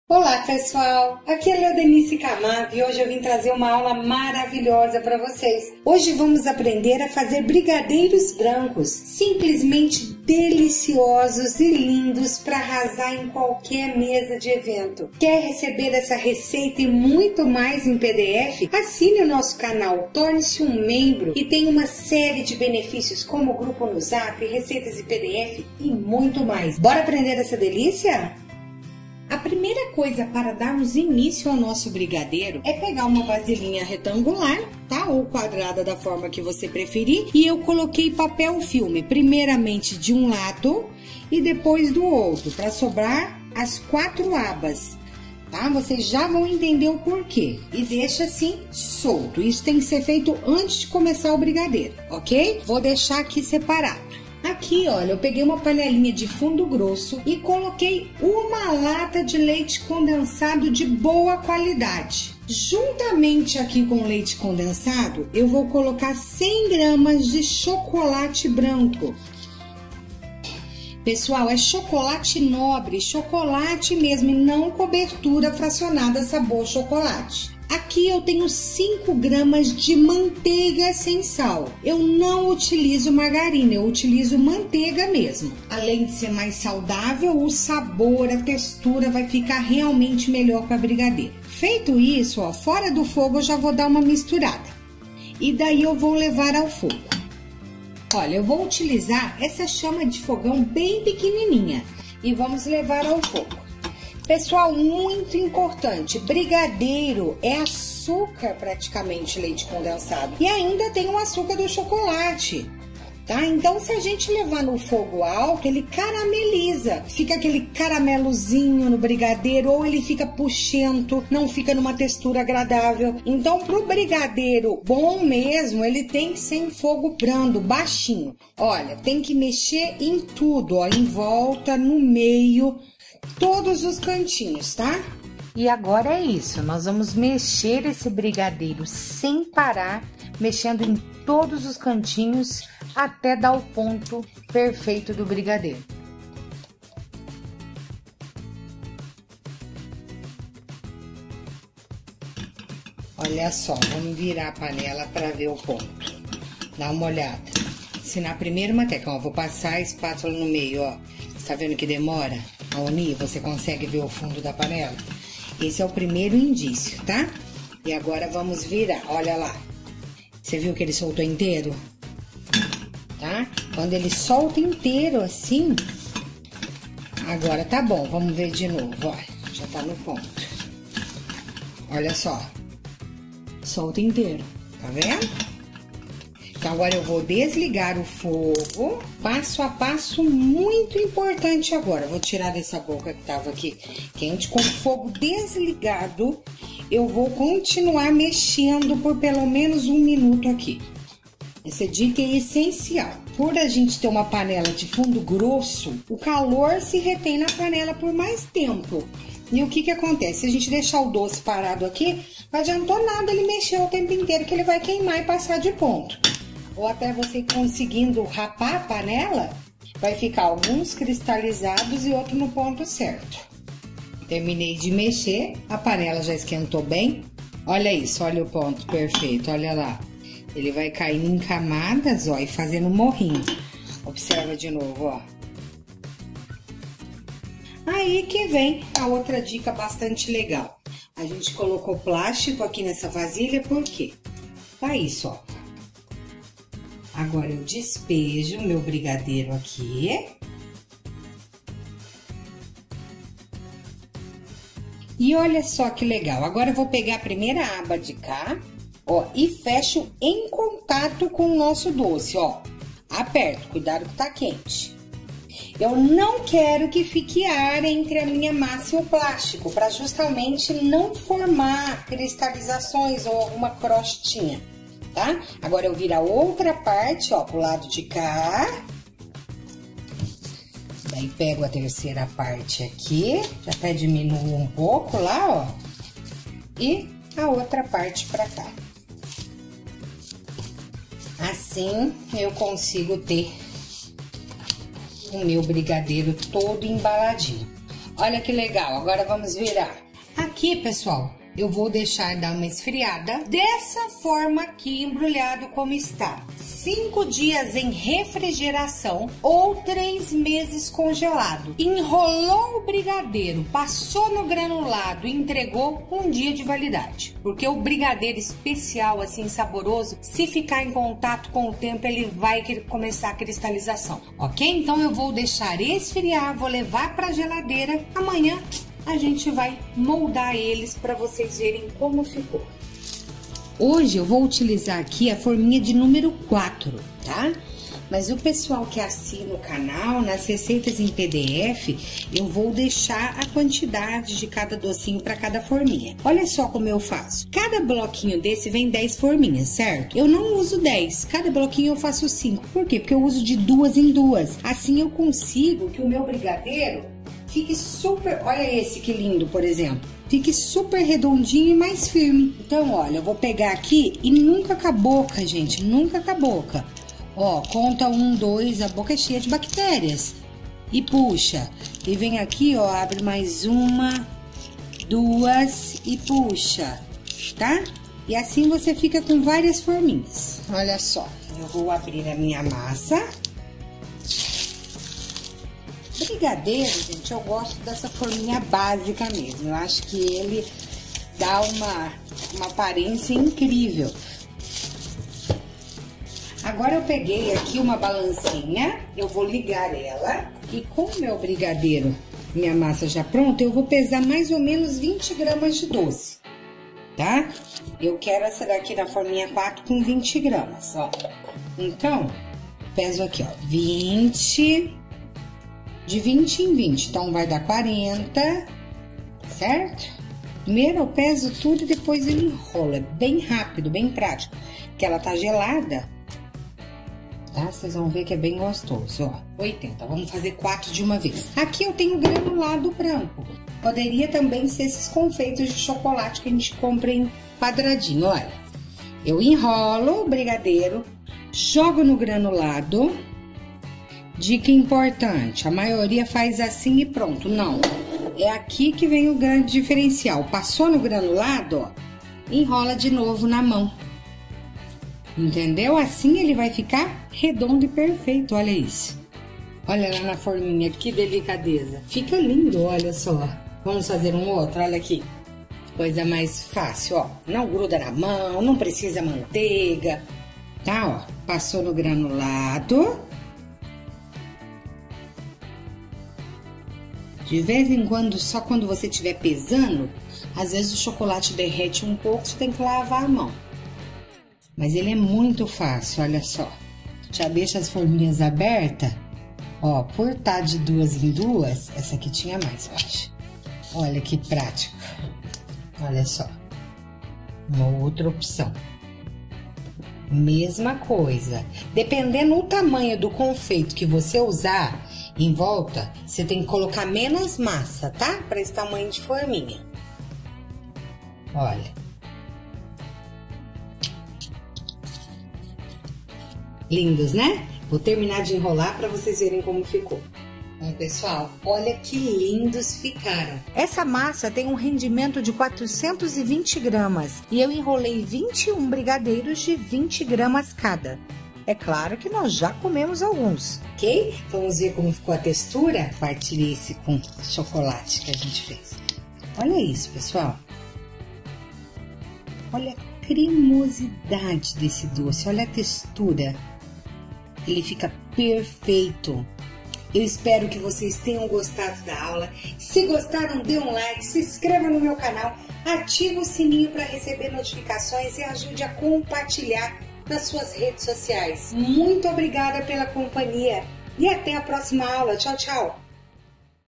BRIGADEIRO BRANCO | SUPER CREMOSO
Hoje vou ensinar esses brigadeiros brancos super cremosos e deliciosos, bora conferir! 😉💜

📦 Para Envios de Correspondências e Entregas:
Leodenice Fernandes Camargo
Caixa Postal 507
Três Lagoas / MS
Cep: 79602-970

INGREDIENTES:

- 395g de Leite Condensado
- 100g de Chocolate Branco
- 5g de Manteiga s/ Sal
- Granulado Branco o quanto baste